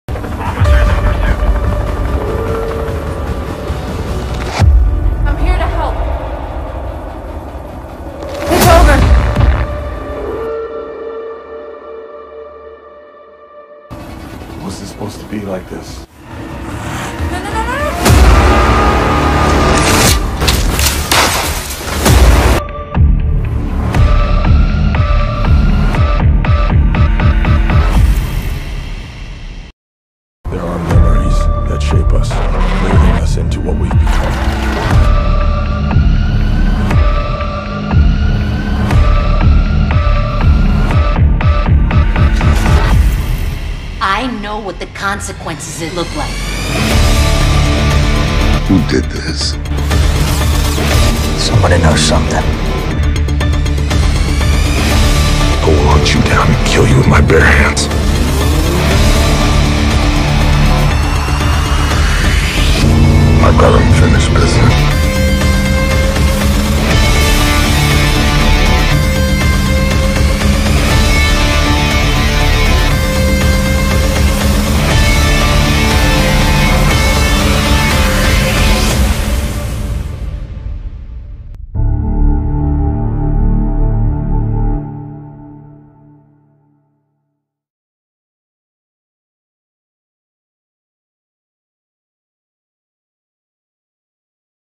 Fast and furious no more family.
so fast and furious no more family this is our final movie for the franchise as we are going into a new movie called no rules which will be coming out next as always we will like and subscribe